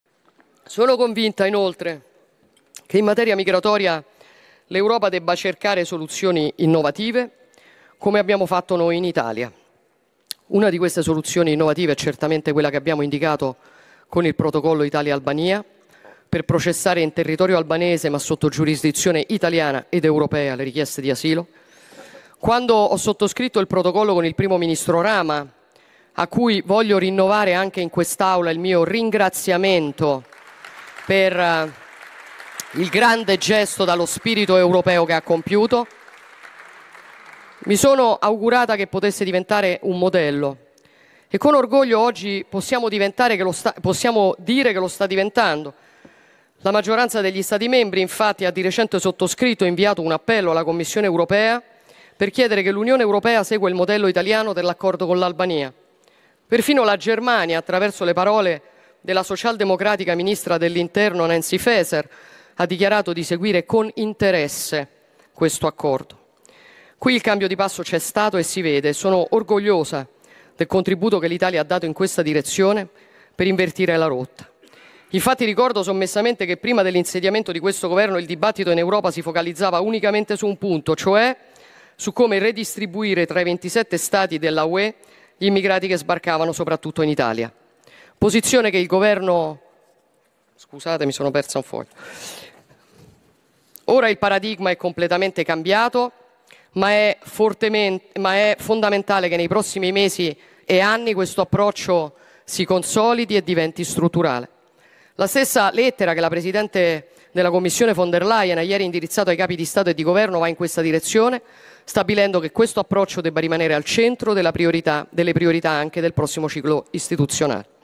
Meloni: "Nella lettera di von der Leyen, i migranti restano la priorità"
"Approccio cambiato, ora va consolidato e reso strutturale"

POLITICA (Roma). Sulla gestione del dossier migranti prima si parlava solo di "redistribuzione", mentre "ora il paradigma è cambiato ma è fondamentale che questo approccio sia consolidato e diventi strutturale: la stessa lettera che la presidente della Commissione von der Leyen ha ieri indirizzato ai capi di Stato e di governo va in questa direzione, stabilendo che questo approccio debba rimanere al centro anche delle priorità anche del prossimo ciclo istituzionale". Così la presidente del Consiglio Giorgia Meloni alla Camera nelle comunicazioni in vista del Consiglio europeo. (NPK) (Emilio Moretti/alanews)

sono convinta inoltre che in materia migratoria l'Europa debba cercare soluzioni innovative come abbiamo fatto noi in Italia una di queste soluzioni non Certamente quella che abbiamo indicato con il protocollo italia-albania per processare in territorio albanese Ma sotto giurisdizione italiana Ed Europea le richieste di asilo quando ho sottoscritto il volo con il primo ministro Rama a cui voglio rinnovare anche in quest'aula Il mio ringraziamento per il grande gesto dallo spirito europeo che ha compiuto mi sono augurata che potesse diventare un modello e con orgoglio Oggi possiamo diventare che lo possiamo dire che non sta diventando la maggioranza degli Stati membri Infatti a dire 100 sottoscritto inviato un appello alla commissione europea per chiedere che l'Unione Europea se quel modello italiano dell' accordo con l'Albania perfino la Germania attraverso le parole della socialdemocrazia  Ministro dell'Interno Nancy fesr ha dichiarato di seguire con interesse questo accordo qui il cambio di Passo c'è stato e si vede sono orgogliosa del contributo che l'Italia in questa direzione per invertire la rotta Infatti ricordo sommessamente che prima dell'insediamento di questo governo il dibattito in Europa si focalizzava unicamente su un punto cioè su come redistribuire 326 stati della Ue gli immigrati che sbarcavano soprattutto in Italia posizione che il governo Scusate mi sono perso  Ora il paradigma è completamente cambiato ma è fortemente ma è fondamentale che nei prossimi mesi e anni questo approccio si consolidi e diventi strutturale la lettera che la presidente della commissione von der Liar indirizzato ai capi di stato e di governo va in questa direzione stabilendo che questo approccio debba rimanere al centro della priorità delle priorità anche del prossimo ciclo Istituto